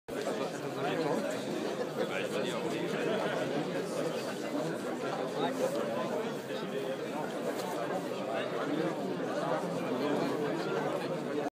Débrayage chez Thyssen
Débrayage chez Thyssen vendredi 26 septembre après l'annonce d'un plan social le 25 près d'Angers